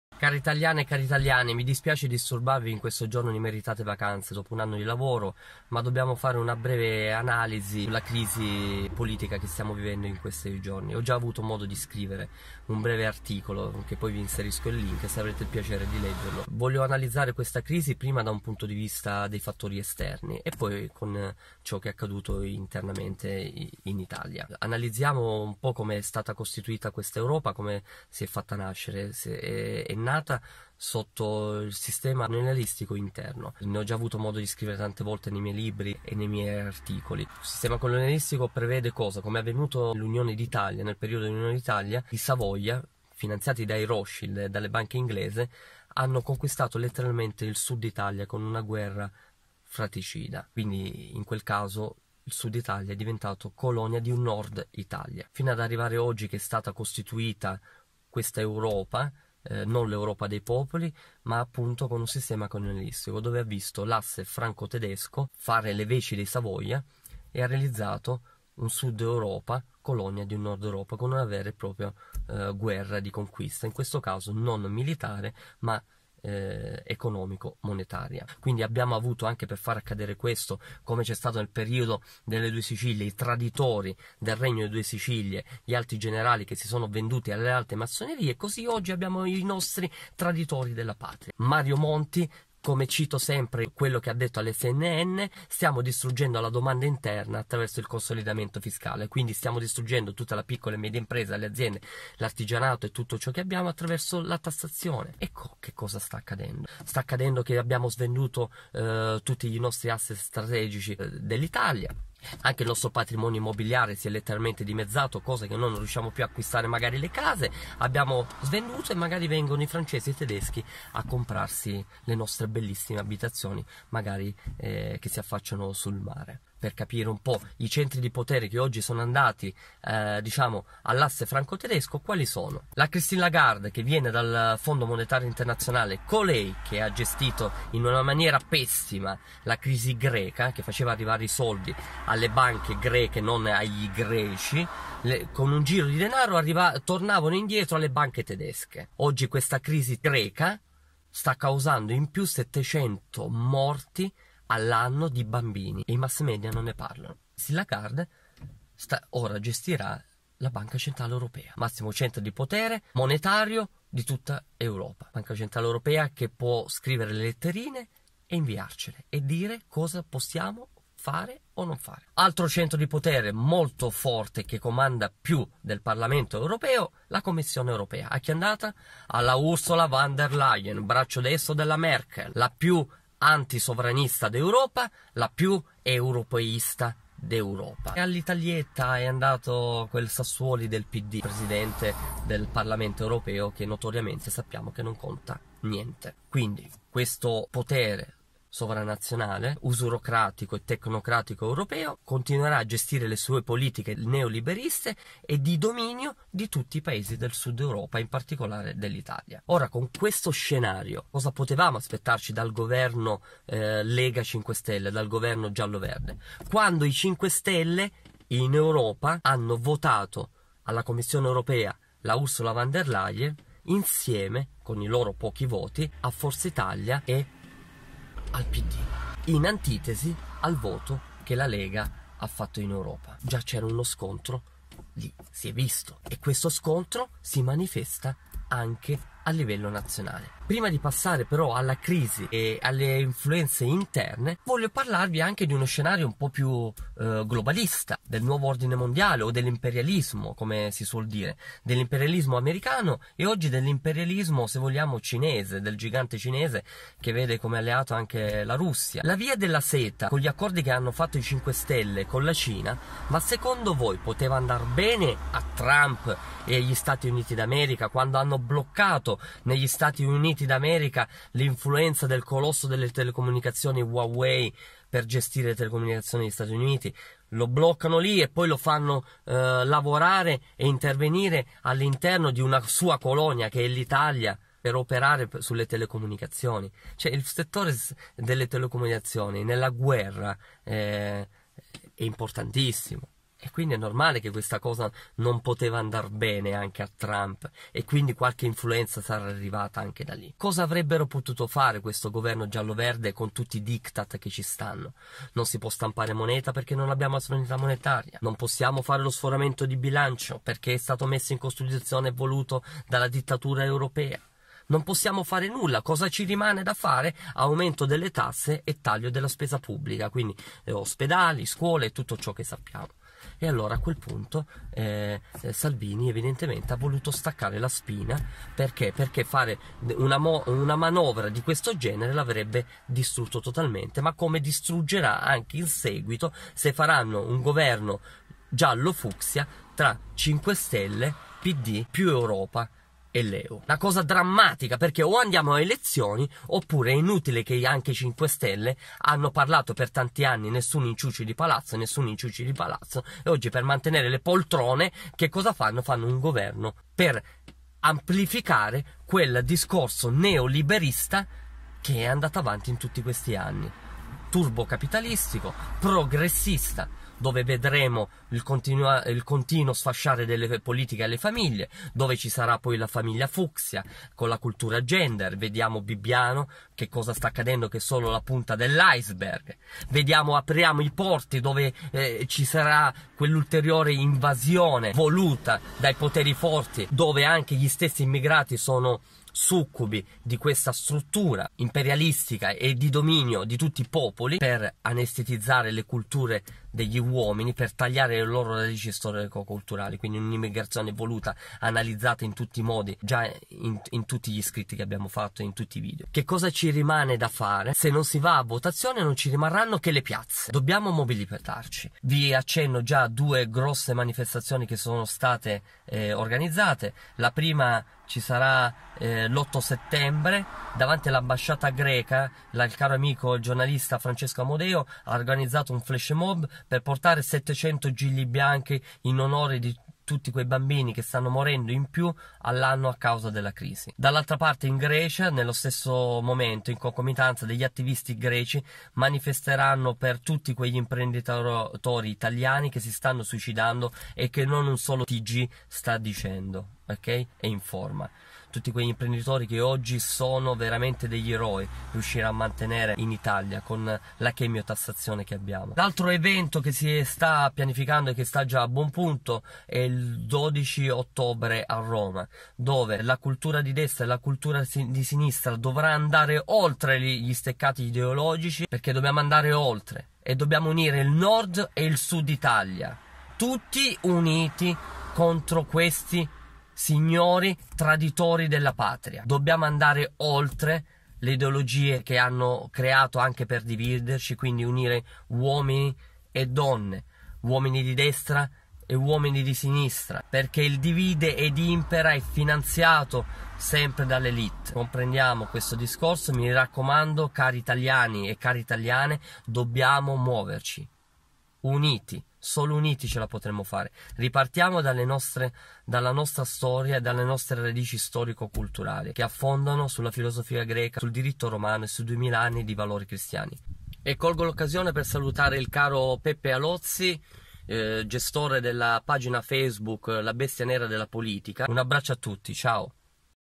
Crisi di Governo-Cosimo Massaro
“Dobbiamo unire il Nord ed il Sud Italia,
tutti uniti,dobbiamo andare oltre le ideologie che hanno creato per dividerci,
perché il ‘Divide et Impera’ è finanziato sempre dalle Élite.”

Cosimo Massaro-esperto di politiche monetarie-div./Facilitatore di Verità Scomode-div.del pensiero Auritiano

Se il video ti è piaciuto lascia il tuo like 👍 e condividi